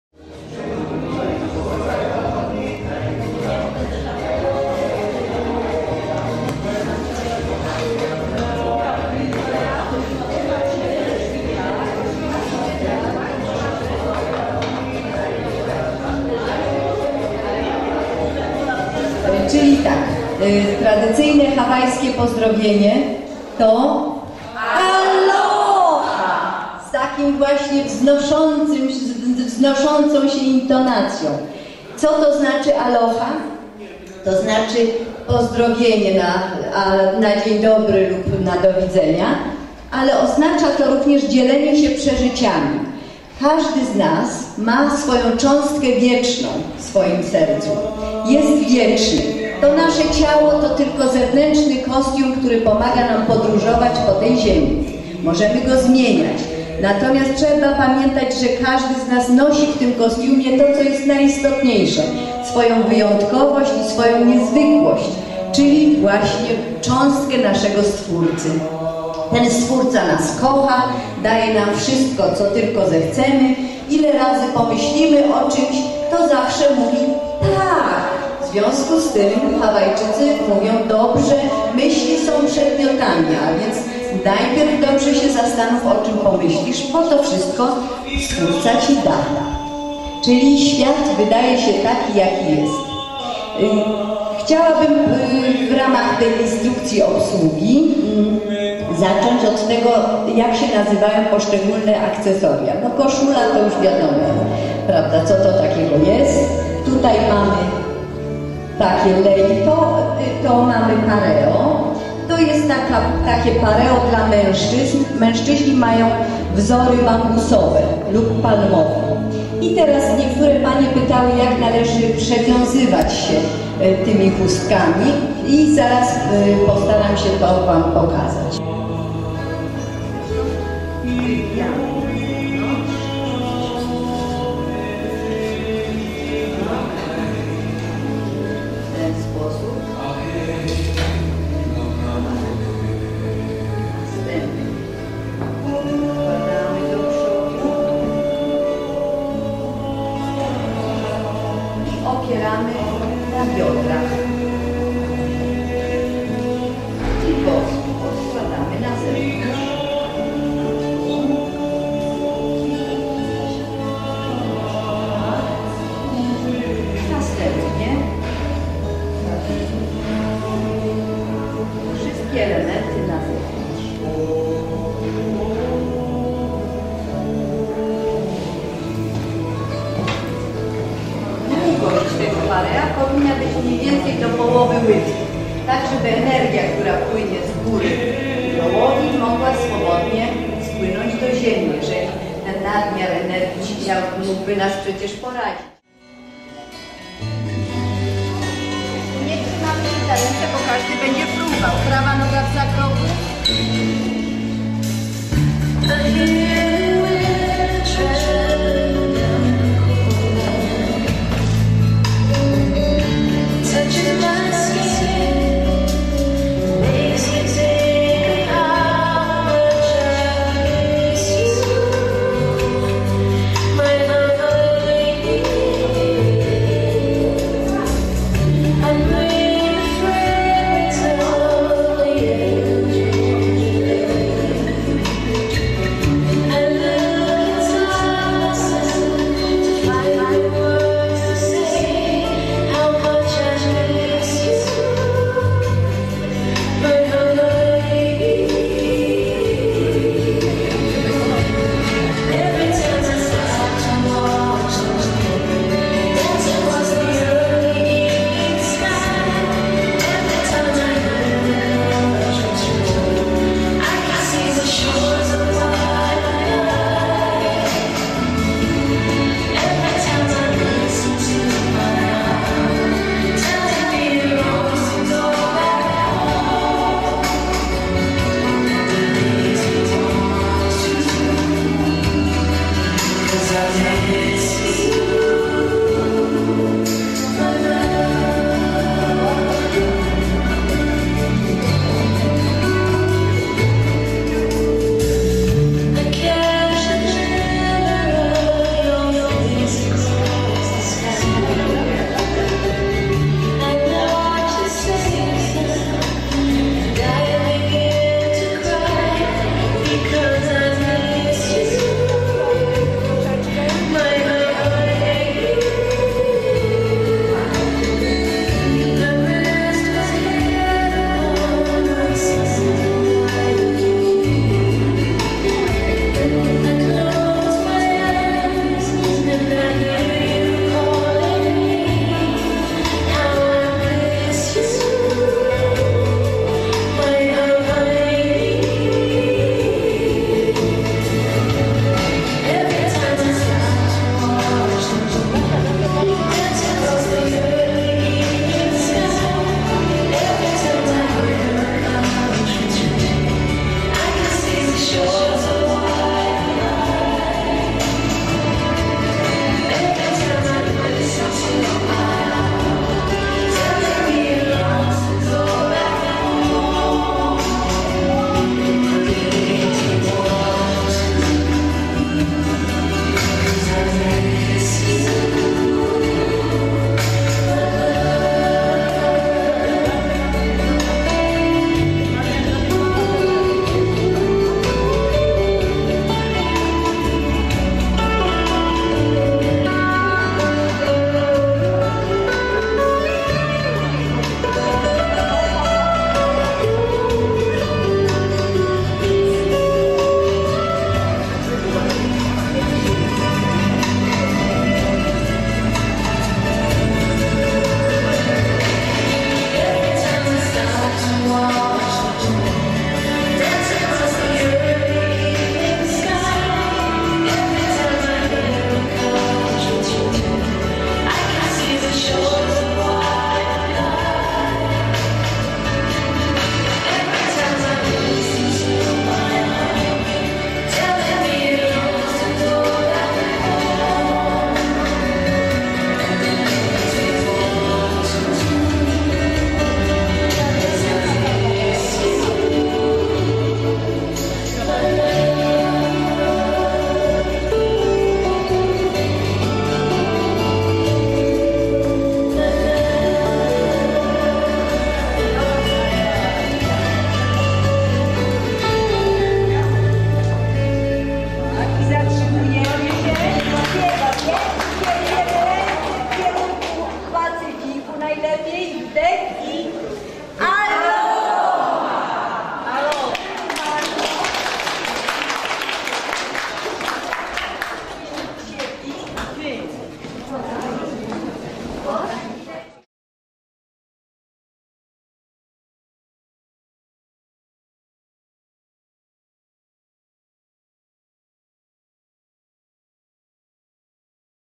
ALOHA FESTIWAL - 2009 Katowice cz.1.
Huna to starożytny system wiedzy psychologicznej i socjologicznej obejmujący wyjątkowo skuteczne praktyki poznania pozazmysłowego, uzdrawiania, czy kreacji. Huna to też styl życia oparty na powszechnej życzliwości i miłości, na doskonałej harmonii. Samo słowo Huna oznacza głębsze, ukryte znaczenie. Huna zadziwiająco sprawdza się w naszej rzeczywistości. Okruchy wiedzy przetrwały w różnych kulturach i stanowią dziś osnowę rożnych szkół rozwoju nie tylko duchowego. Praktykowanie Huny prowadzi do stanu wewnętrznej harmonii ze wszystkim co nas otacza, z samym sobą i całym światem. Jednocześnie urzeczywistniając swoje pragnienia w duchu Huny zmieniamy nie tylko siebie, ale też otaczającą nas rzeczywistość. Zaczynamy tak naprawdę się rozwijać...